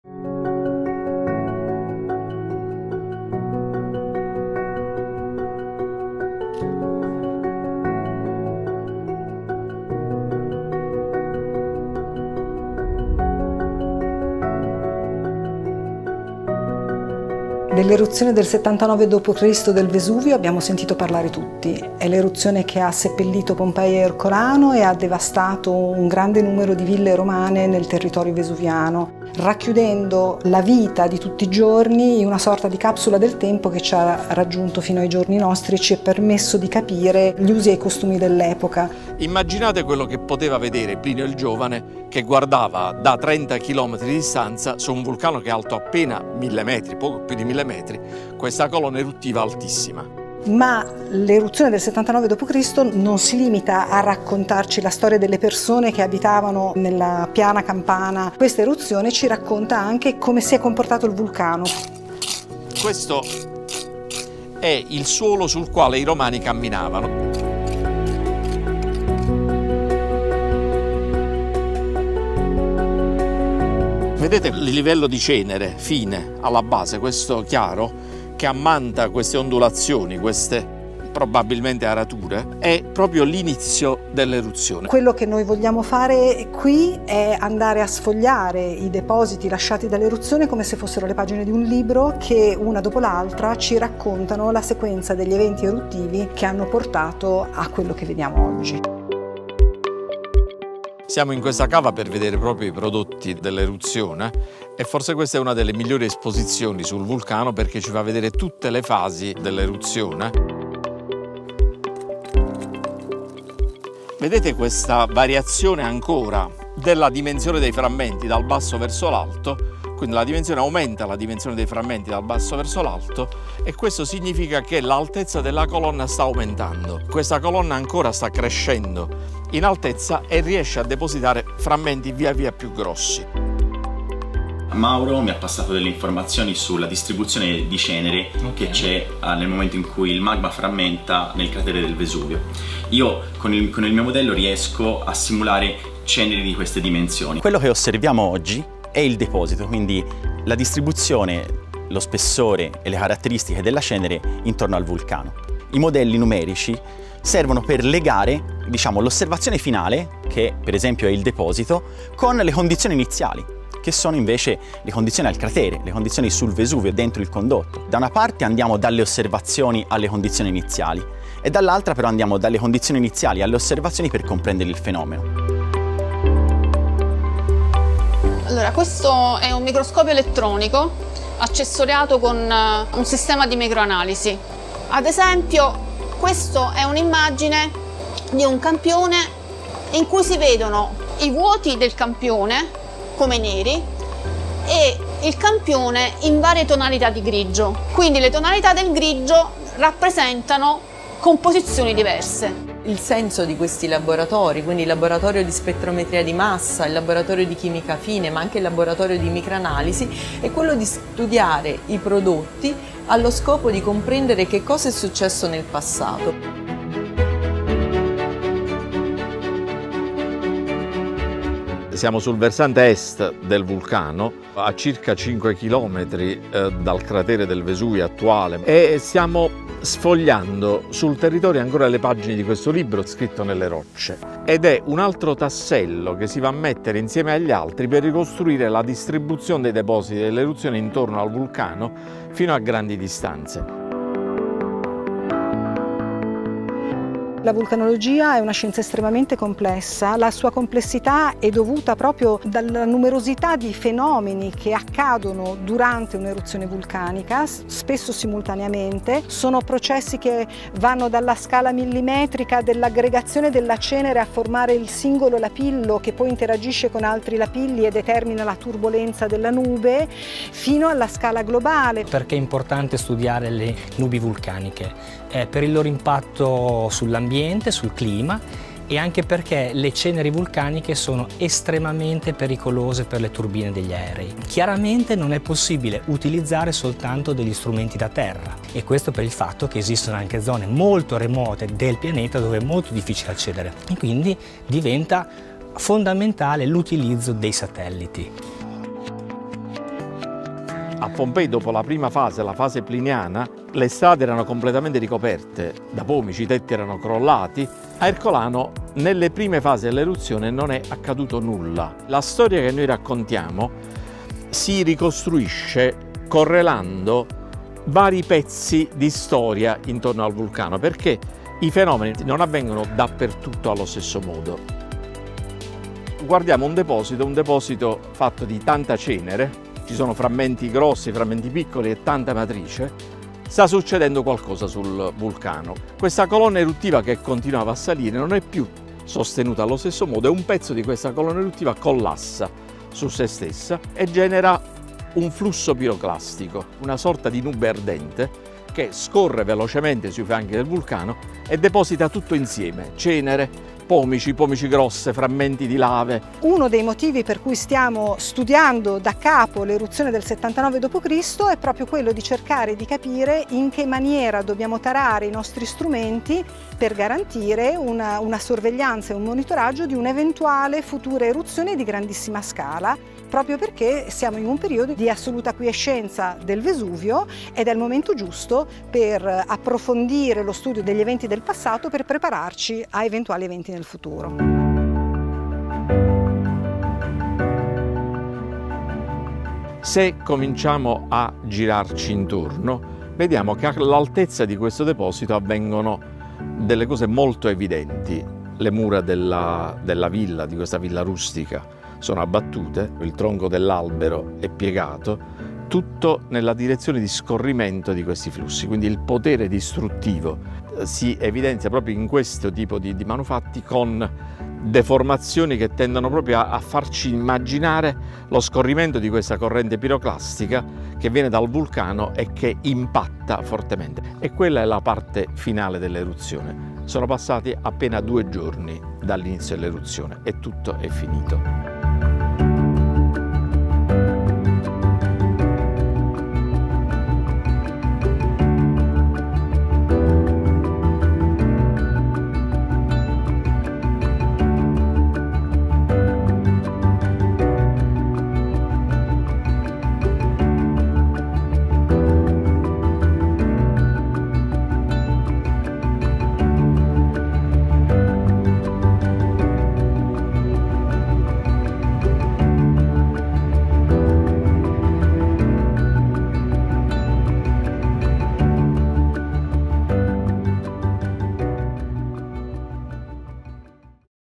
Pompei 79dC - Studio di un'eruzione
Cosa succederebbe se ci fosse un'eruzione di grande scala? Saremmo  pronti? Per saperlo, bisogna conoscere bene  questi grandi eventi, la loro dinamica e i loro effetti. Volendo approfondire la conoscenza delle grandi eruzioni, da dove partire se non dallo studio della più famosa fra tutte?
Questo video racconta di un progetto finanziato dall'INGV e interamente dedicato allo studio delle grandi eruzioni, a partire da quella famosissima del 79 dC.

Hanno contribuito ricercatrici e ricercatori delle sezioni INGV di Napoli - Osservatorio Vesuviano, Roma - ONT, Pisa e Bologna.
Le riprese sono state effettuate a Villa Regina (Boscoreale), nei locali dell'Osservatorio Vesuviano e presso la sua sede storica (Reale Osservatorio Vesuviano).
Si ringrazia il Parco Nazionale del Vesuvio, il Comune di Boscoreale, il Parco Archeologico di Pompei e la Dottoressa Anna Maria Sodo.

Riprese e montaggio di Lino Greco, con l'assistenza di Agata Greco.
Coordinamento, scrittura e supervisione di Francesca Conti (formicablu)